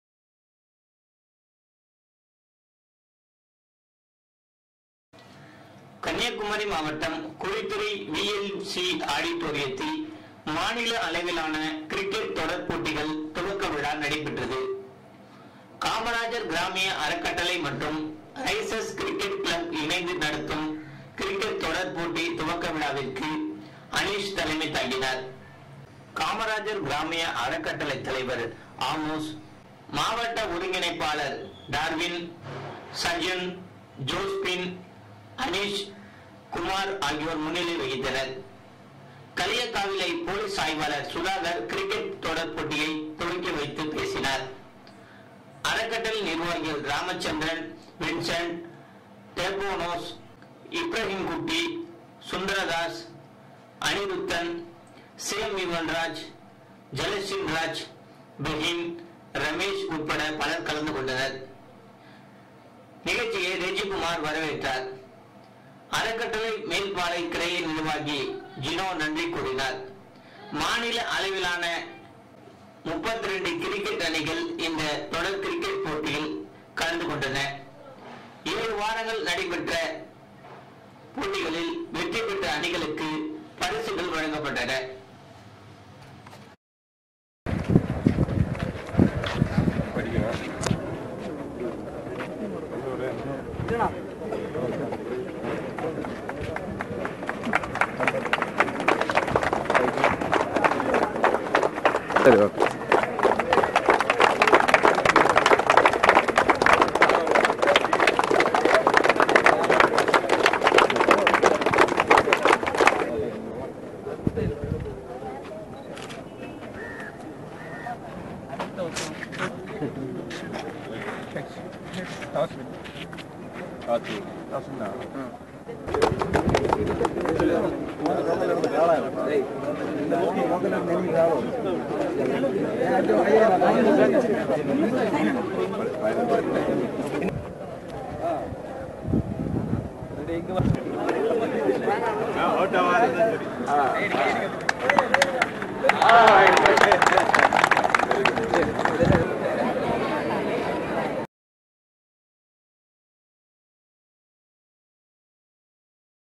kgf cricket match t.t.tv
கன்னியாகுமரி, குழித்துறையில் காமராஜர் கிராமிய அறக்கட்டளை நடத்திய மாநில அளவிலான கிரிகட் போட்டி.